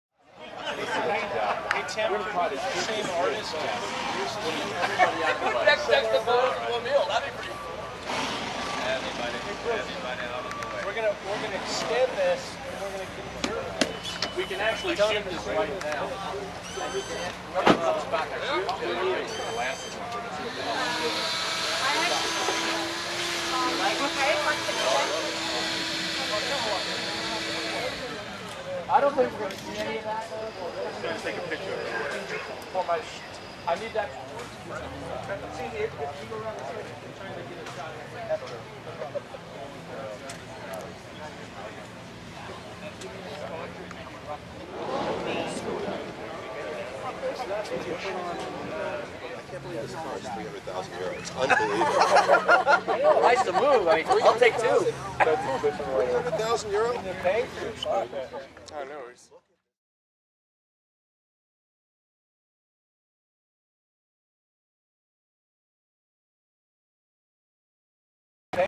Bugatti Vitesse 'Legends' Jean-Pierre Wimille Edition!
This video features the new Bugatti Veyron Vitesse 'Legends' Jean-Pierre Wimille Edition, 1 of 6 Legends to be unveiled this year. It's made up solely of blue carbon and a light blue paint and looks absolutely stunning in person. Enjoy the video!

Location: Pebble Beach, CA